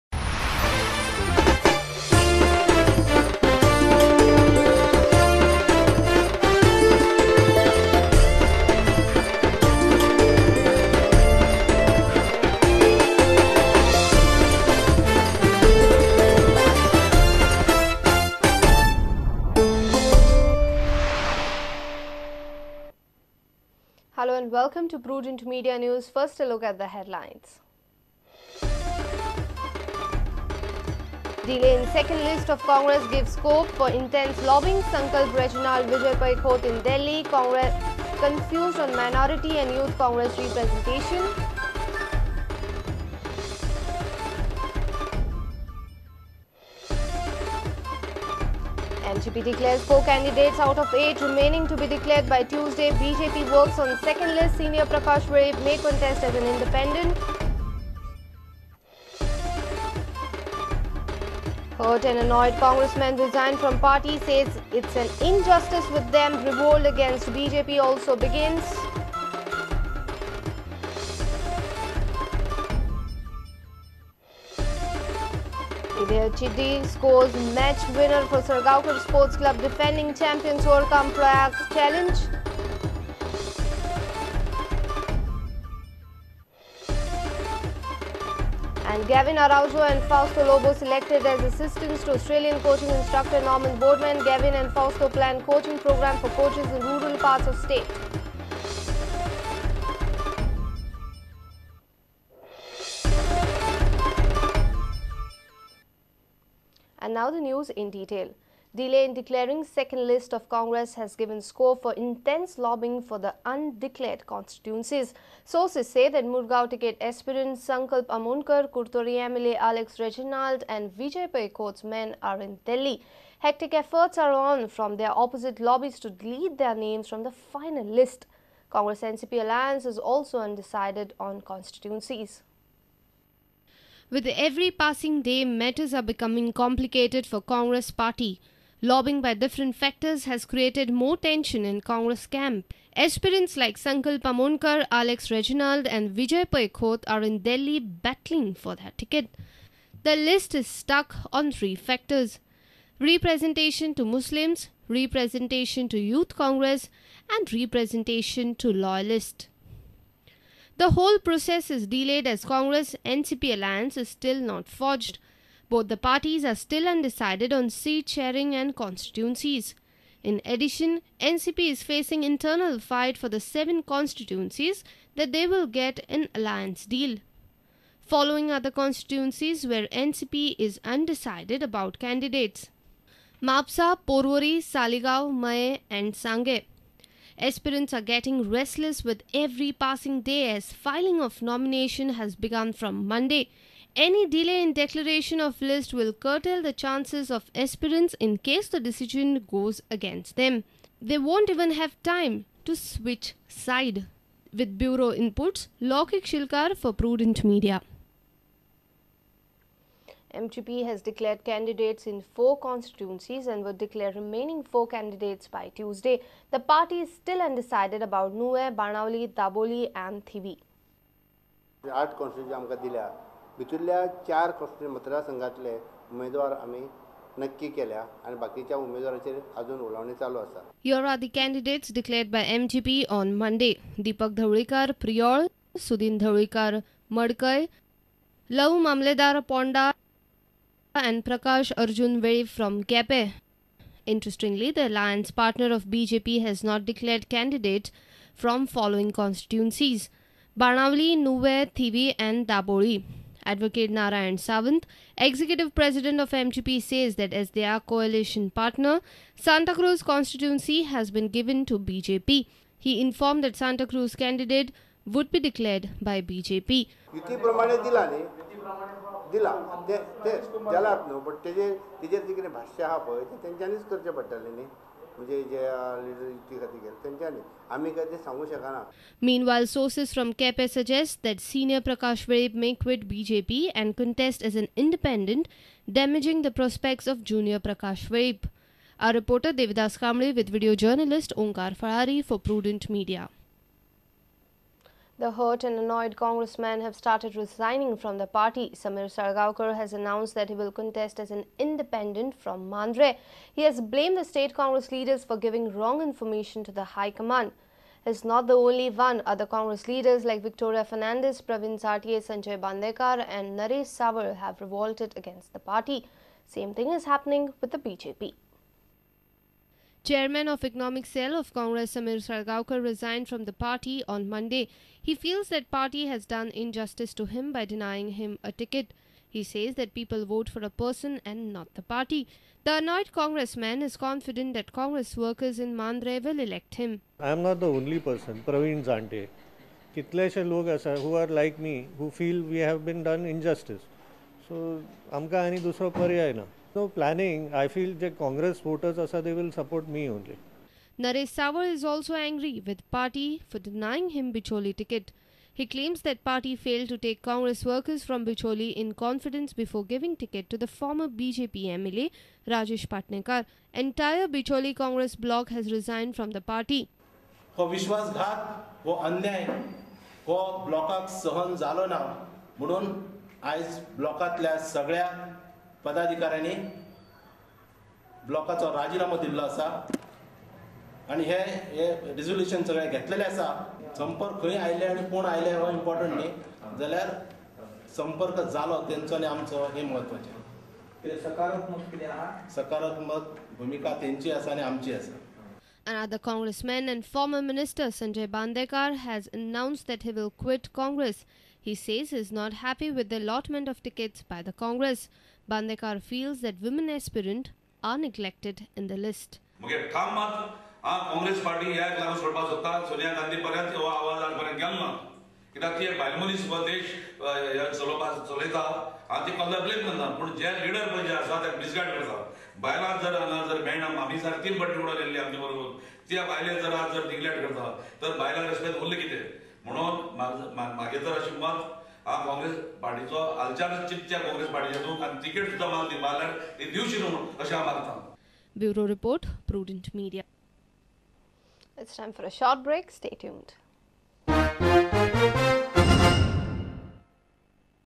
Prudent Media English News 06 Feb 12 Part 1
Delay in 2nd list of Congress gives scope for intense lobbying, Sankalp, Reginald, Vijay Pai khot in Delhi, Cong confused on minority and youth Congress representation